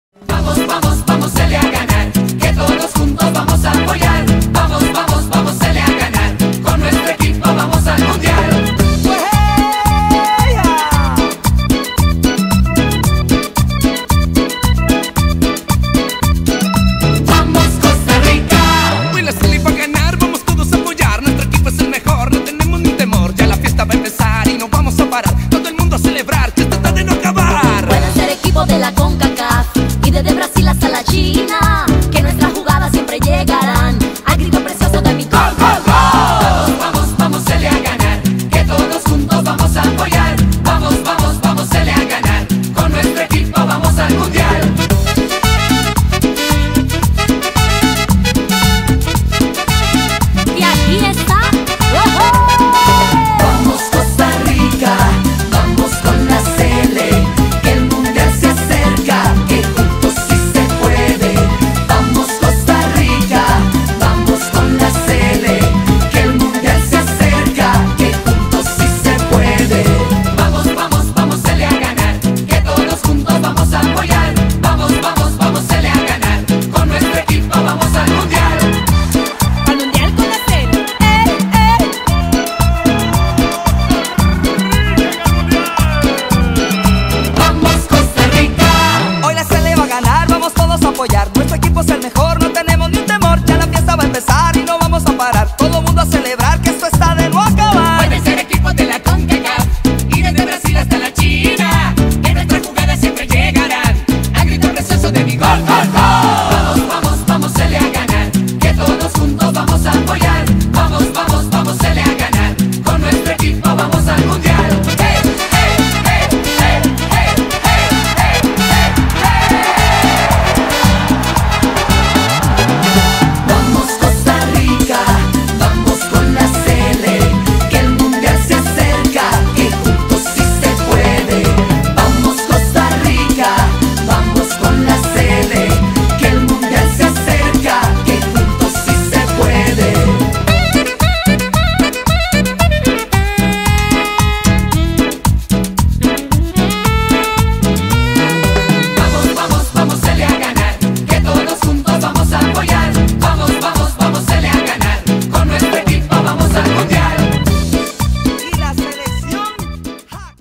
Homenaje a José Miguel Cubero
Un jugador 100% Sarchiseño, orgullos del 22 de la sele